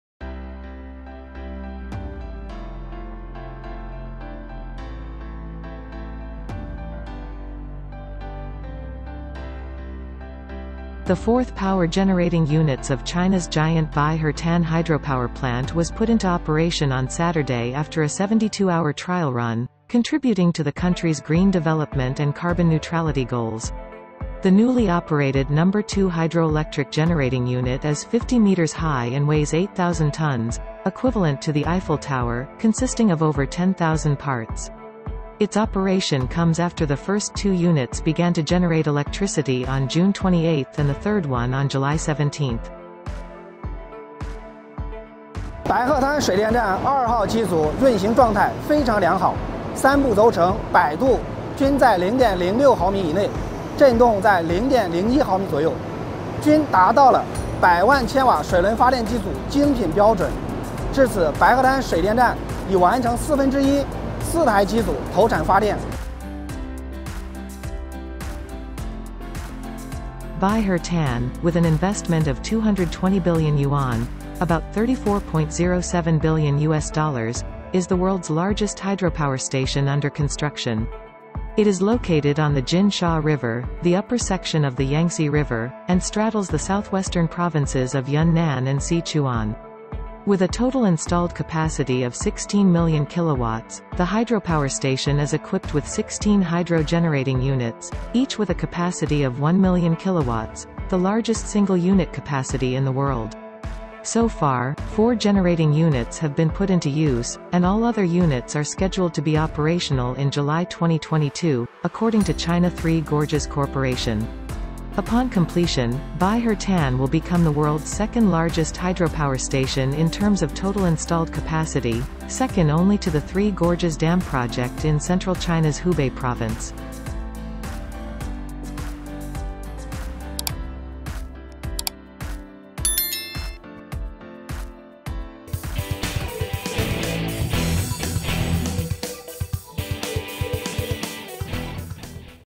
The fourth unit of China's Baihetan mega hydropower station was put into operation | 中国特大型水电站第四台机组投运
The fourth power-generating units of China's giant Baihetan hydropower plant was put into operation on Saturday after a 72-hour trial run, contributing to the country's green development and carbon-neutrality goals.

The newly-operated No. 2 hydroelectric generating unit is 50 meters high and weighs 8,000 tons, equivalent to the Eiffel Tower, consisting of over 10,000 parts. Its operation comes after the first two units began to generate electricity on June 28 and the third one on July 17.

"The No. 2 Unit of the Baihetan hydropower station is in very good operational condition. The swing of the three bearings is within 0.06 mm and the vibration is about 0.01 mm, reaching the high-quality standard of water turbine power-generating units that can generate one million kilowatts. So far, a quarter of the construction of the Baihetan Hydropower Station has been completed with four generating units in operation," said Jin Kun, deputy head of the Electrical and Mechanical Project Unit with the Baihetan Engineering Construction Department under the China Three Gorges Corporation which built the station.

Baihetan, with an investment of 220 billion yuan (about 34.07 billion U.S. dollars), is the world's largest hydropower station under construction. It is located on the Jinsha River, the upper section of the Yangtze River, and straddles the southwestern provinces of Yunnan and Sichuan.

With a total installed capacity of 16 million kilowatts, the hydropower station is equipped with 16 hydro-generating units, each with a capacity of one million kilowatts, the largest single-unit capacity in the world.

So far, four generating units have been put into use, and all other units are scheduled to be operational in July 2022, according to China Three Gorges Corporation.

Upon completion, Baihetan will become the world's second-largest hydropower station in terms of total installed capacity, second only to the Three Gorges Dam project in central China's Hubei Province.

经过72小时试运行，中国巨型白鹤滩水电站第四台机组于周六投产，为国家的绿色发展和碳中和目标做出了贡献。

新投产的2号水轮发电机组高50米，重8000吨，相当于埃菲尔铁塔，由1万多个零件组成。它的运营是在前两台机组于6月28日和第三台于7月17日开始发电之后进行的。

“白鹤滩水电站2号机组运行状态非常好，三个轴承的摆动在0.06毫米以内，振动在0.01毫米左右，达到百万千瓦水轮发电机组高质量标准。目前，白鹤滩水电站已完成四分之一建设，4台机组投入运行。”建设该站的中国三峡集团公司白鹤滩工程建设部机电项目组副组长金坤说。

投资2200亿元人民币（约合340.7亿美元）的白鹤滩是世界上最大的在建水电站。地处长江上游金沙江畔，横跨云南、四川两省。

水电站总装机容量1600万千瓦，配备16台水轮发电机组，每台容量100万千瓦，是世界上单机容量最大的水电站。

据中国三峡集团公司介绍，截至目前，已有4台机组投入使用，其余机组计划于2022年7月投产。

建成后，按总装机容量计，白鹤滩将成为世界第二大水电站，仅次于中国中部湖北省的三峡大坝项目。